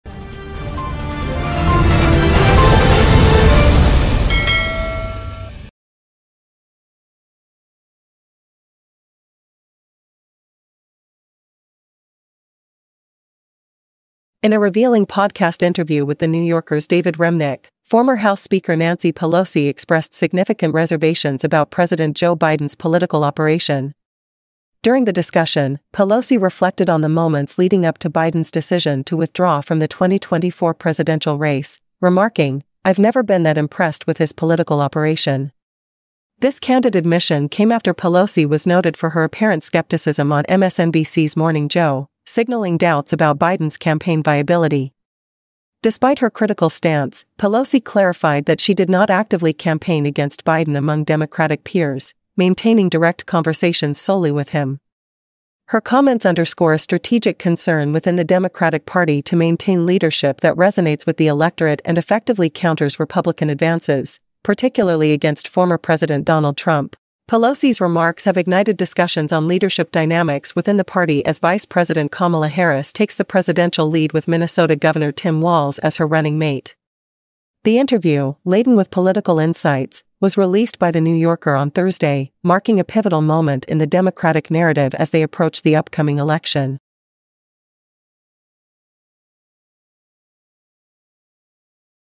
Nancy Pelosi 'Not Impressed': Unveils Discontent with Biden’s Campaign Strategy
Nancy Pelosi, in a startling revelation on The New Yorker Radio Hour, openly criticized President Biden's political strategy, highlighting a lack of confidence in his campaign operations. She detailed her discreet approach to political discussions, focusing directly on Biden, amidst widespread speculation about her influence over his 2024 campaign decision. Pelosi's sharp critique opens a new chapter in Democratic leadership as they gear up to prevent a Trump comeback.

Nancy Pelosi | Joe Biden | political operation | podcast interview | 2024 election | Democratic leadership | Biden campaign | Kamala Harris | Tim Walz | The New Yorker

Category: Politics